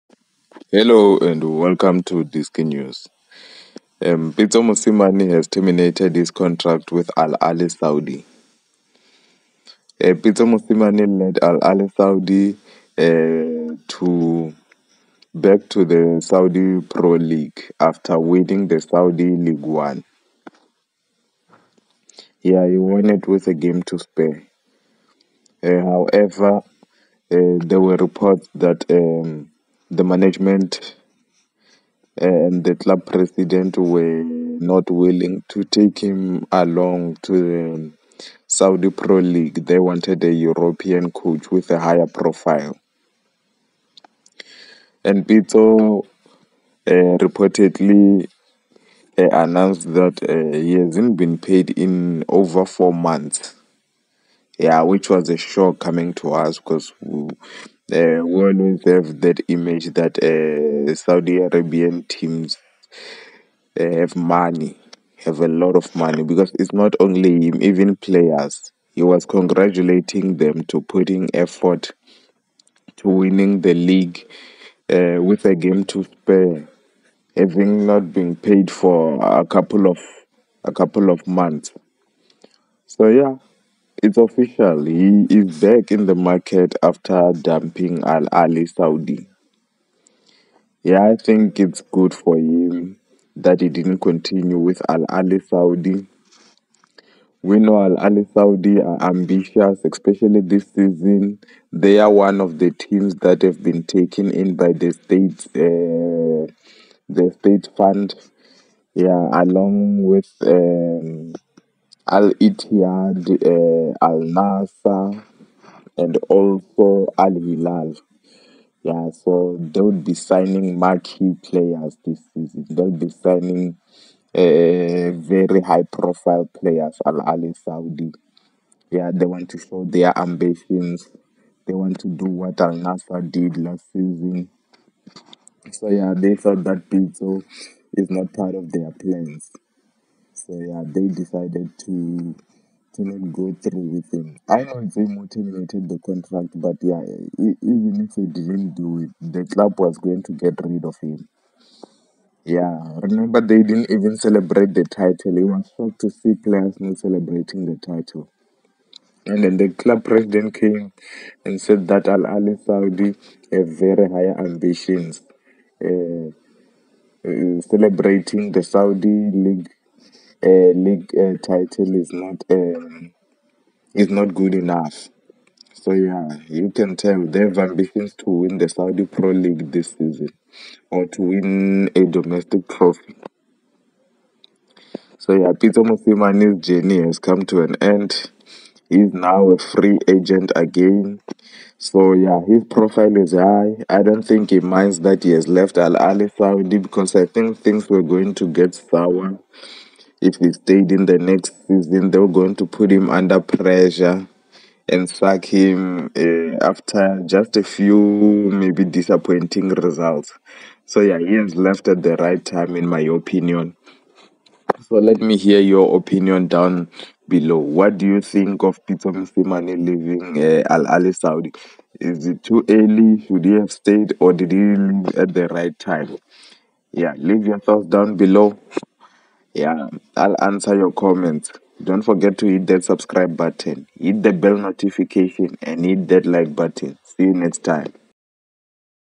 Pitso Mosimane dumps Al Ahli Saudi after not getting paid for 4 months
He led them back to the Saudi Pro League